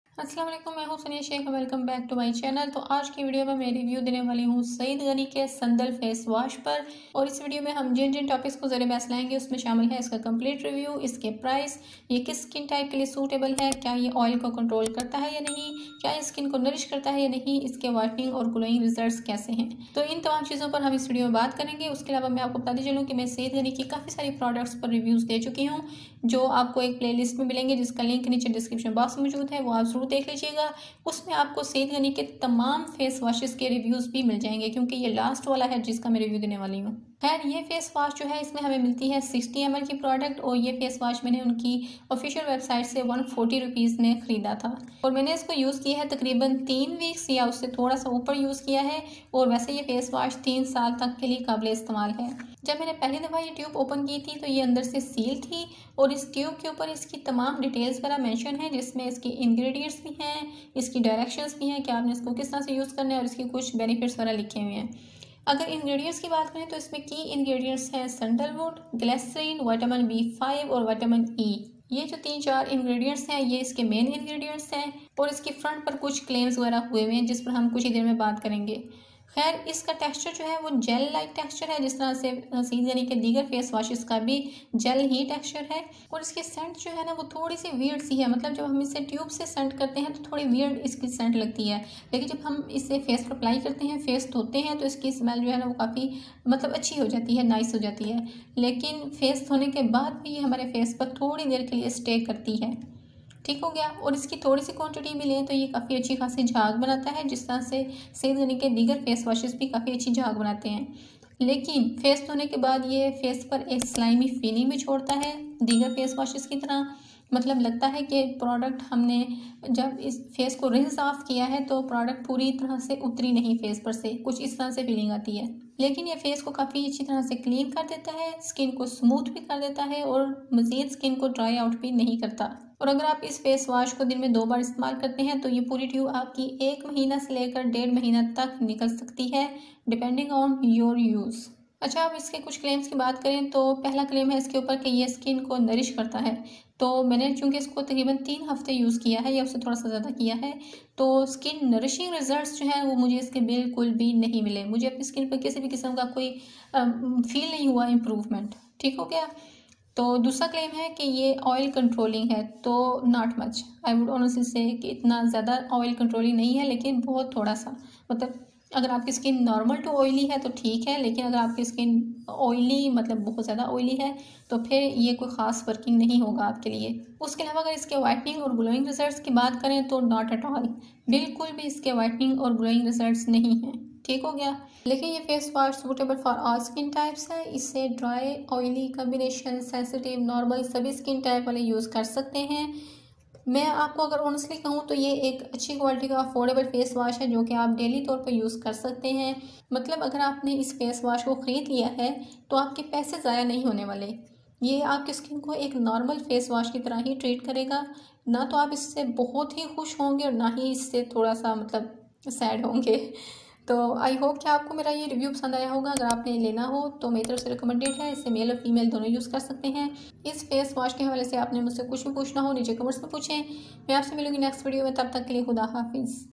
Saeed Ghani | Face Wash | Sandal Face Wash | Review
Today's video about Saeed Ghani Face Wash Review. I tried Saeed Ghani Sandal Face Wash for about 3 weeks and here I am today reviewing this Sandal Face Wash.

Social Links;